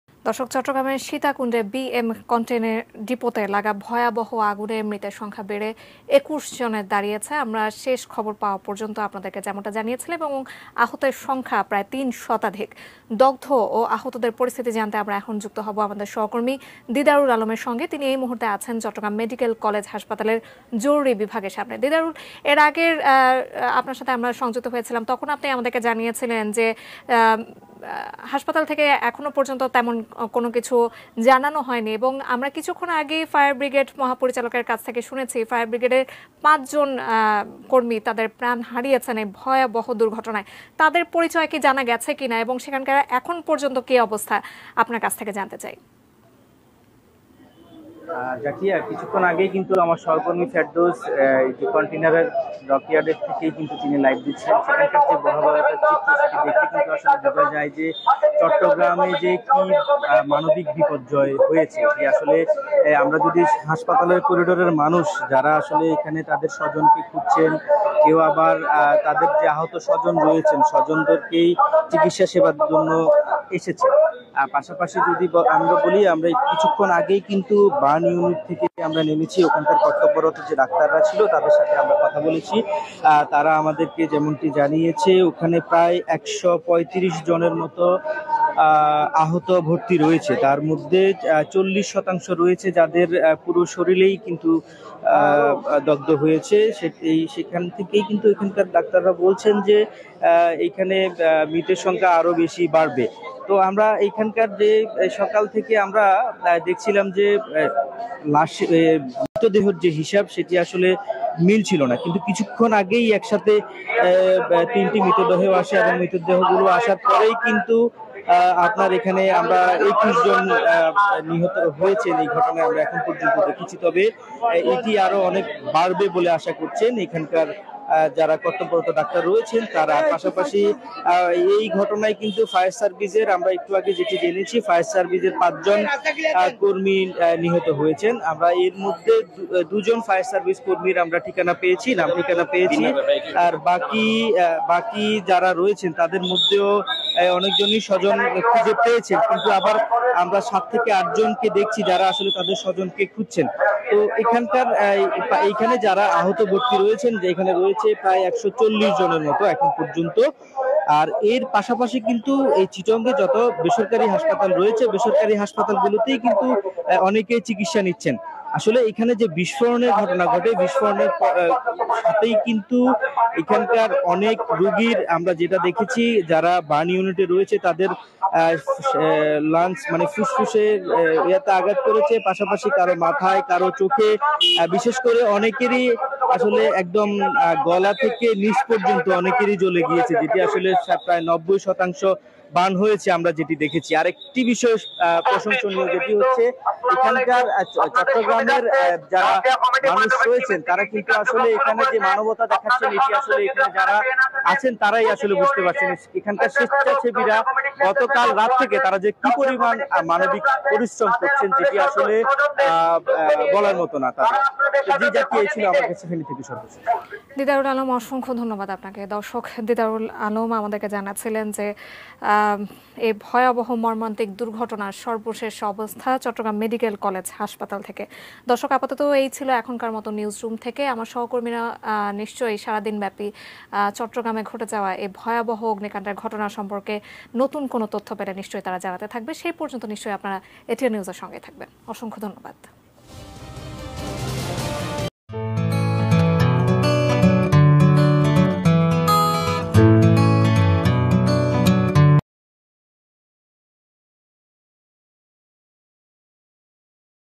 স্বজনদের কান্নার রোল হাসপাতালজুড়ে || ATN News LIVE
আহত অনেকেই চট্টগ্রাম মেডিকেল কলেজ হাসপাতালে, অনেকে খুঁজে ফিরছেন হারিয়ে যাওয়া স্বজনদের..